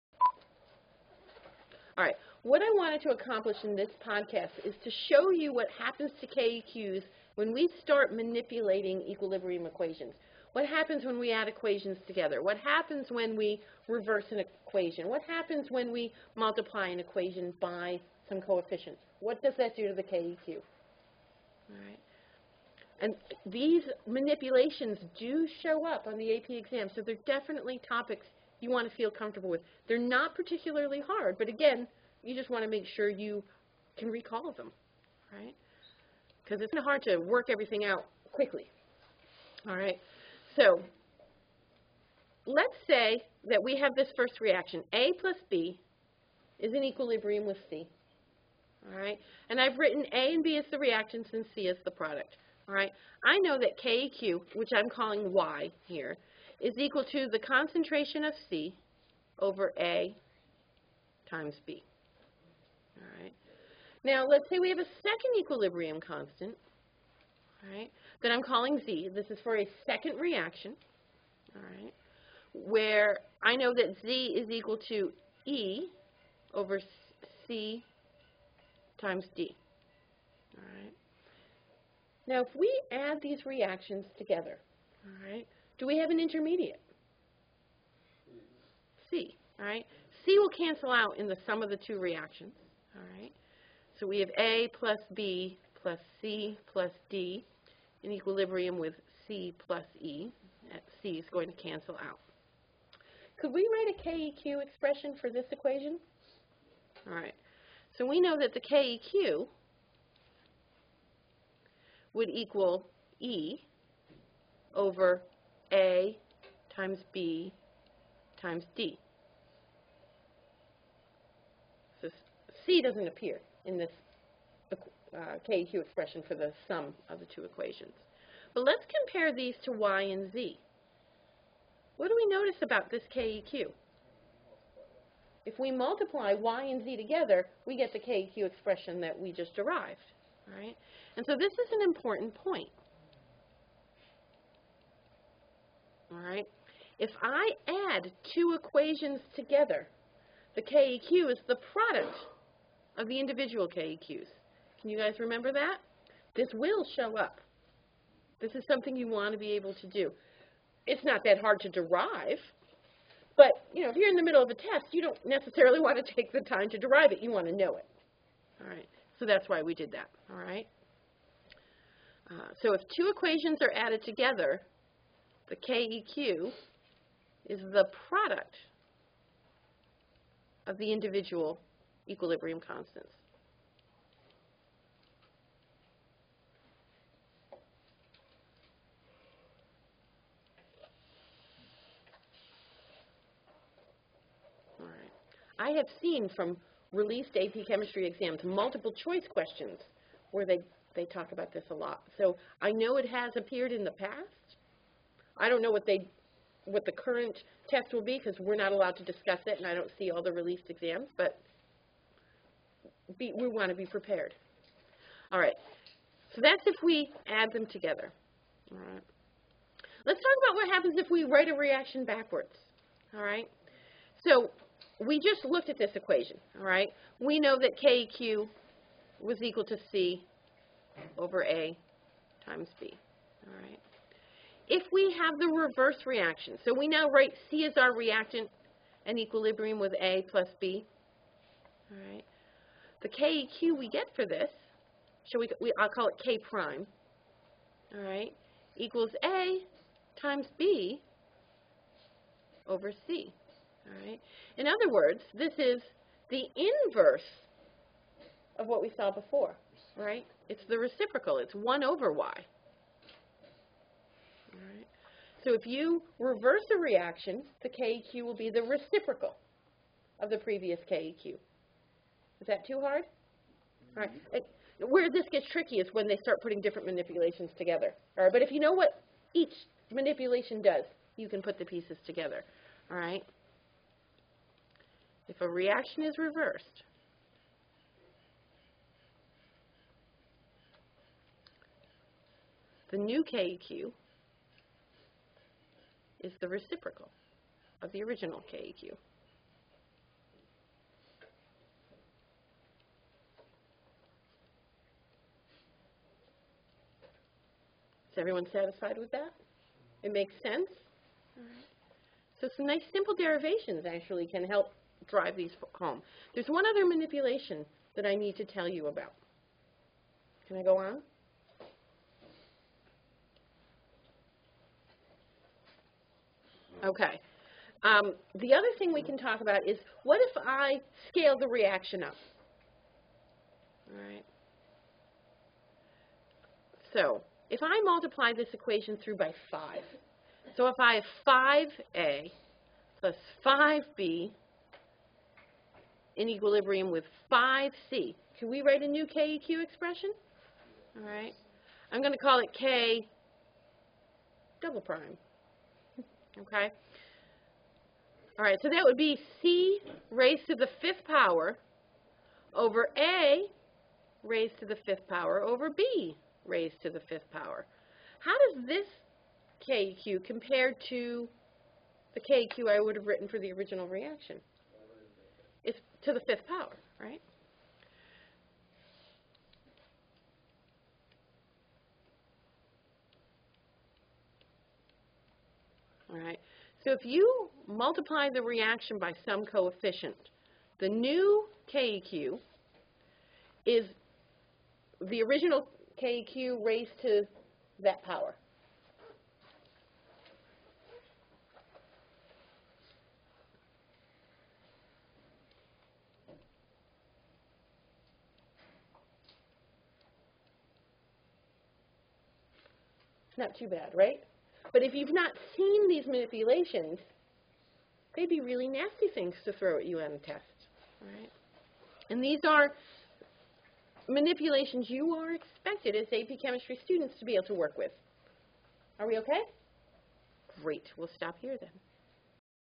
Manipulating Equilibrium Constants
Learn how manipulating equilibrium reactions affects the Keq values.